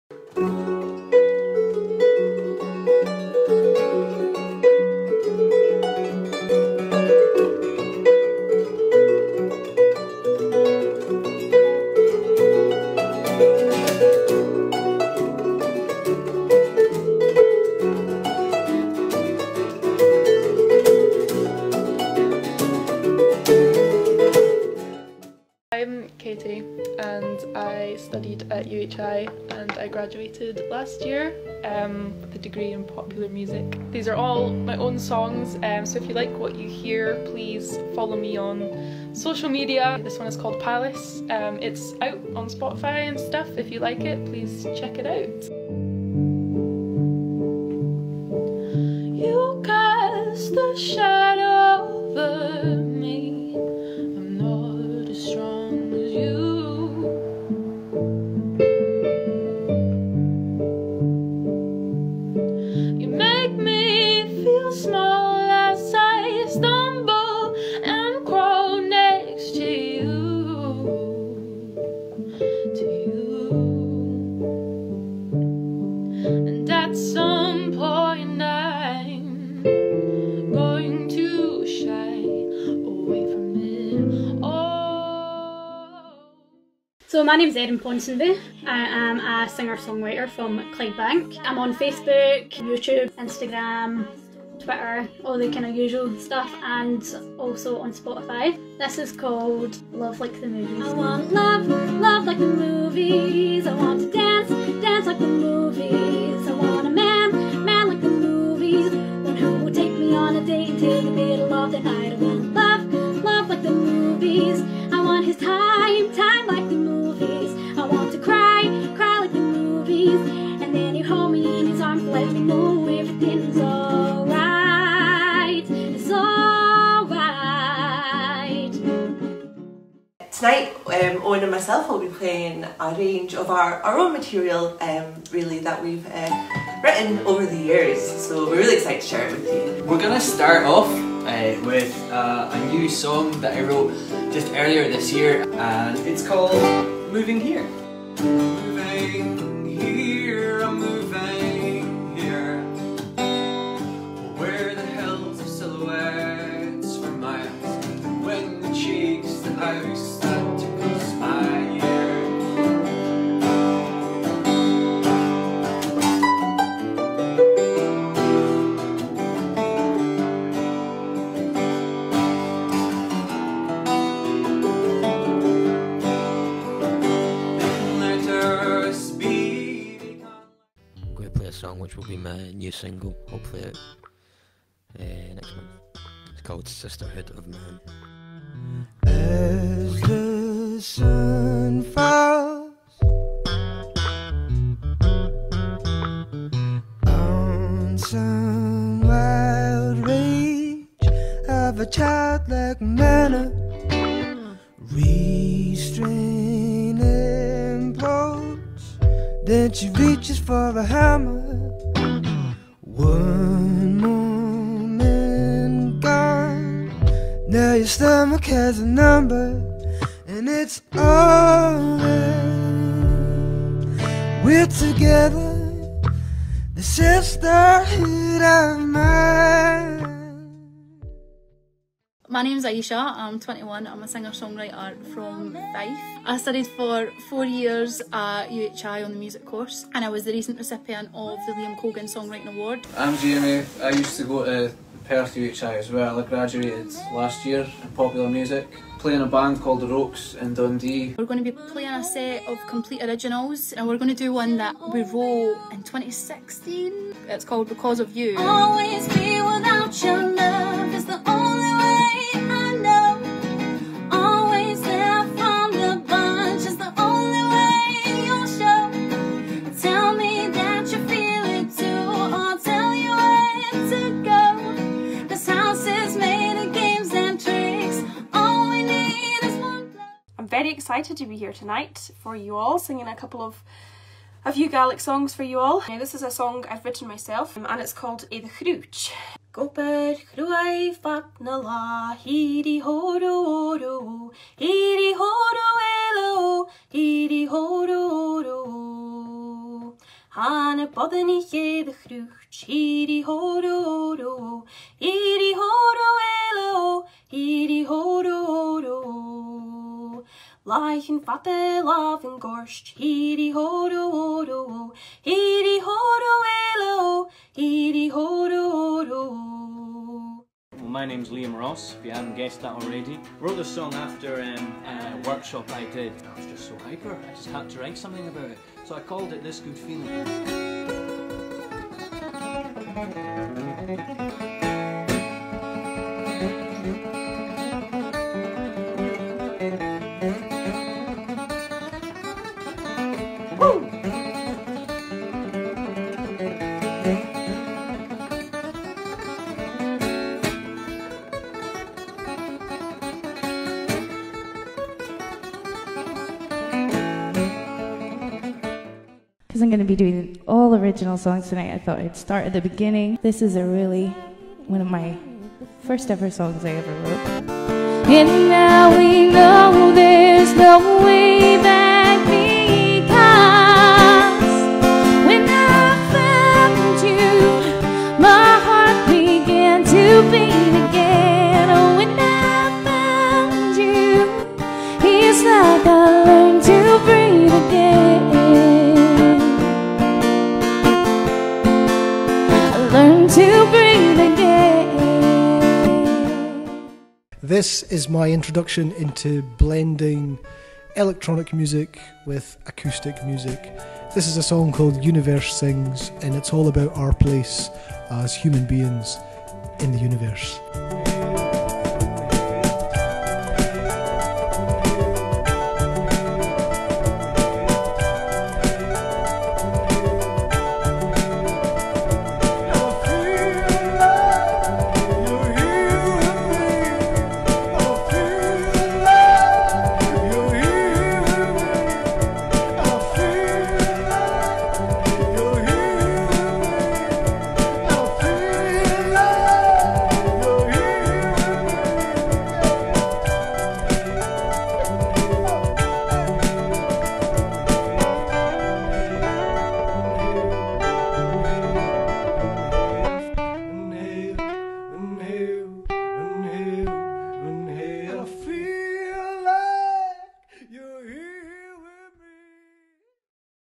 UHIMusicLive Highlights
Highlights of the musical performances from the UHIMusicLive events.

Performers